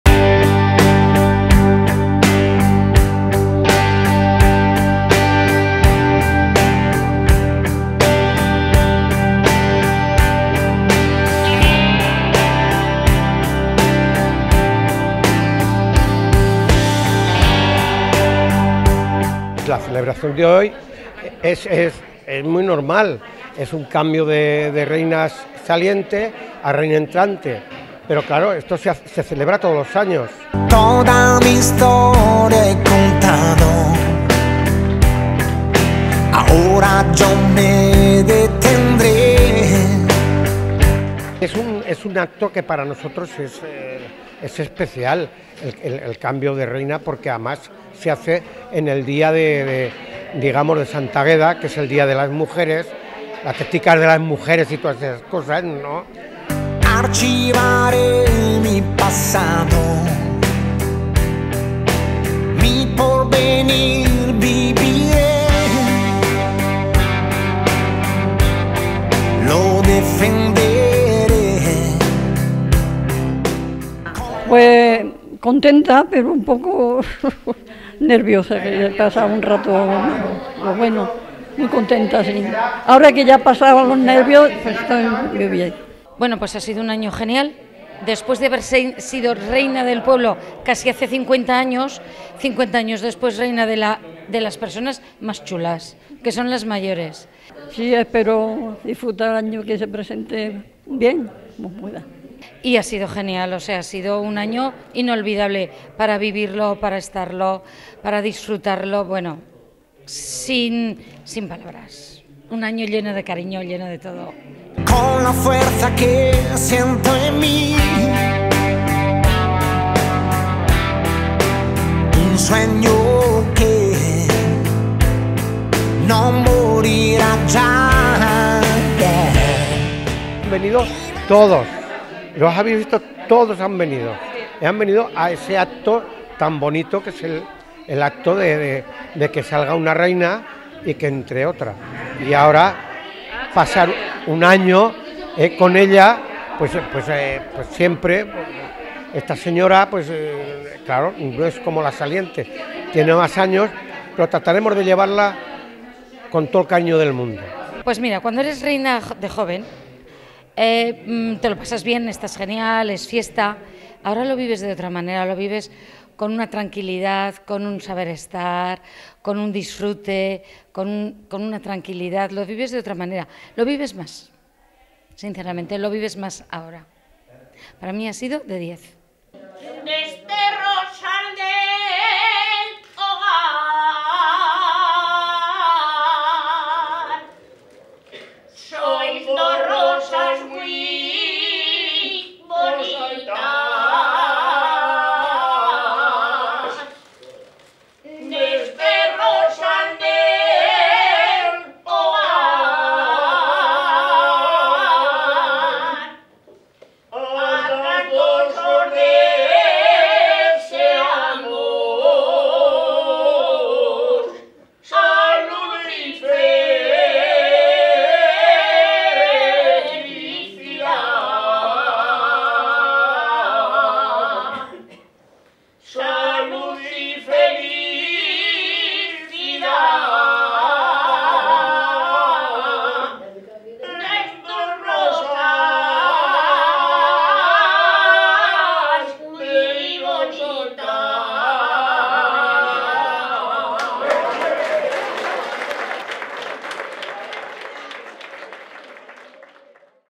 La Comarca.tv - Nueva Reina Hogar Jubilados Calanda
La asociación de jubilados de Calanda celebró durante el día de Santa Águeda el cambio de su reina. Concha Benítez sustituye a María Antonia Fernández para el presente año. María Antonia describé su reinado con una diez en cariño y compañerismo y Concha espera pasarlo muy bien durante el 2014.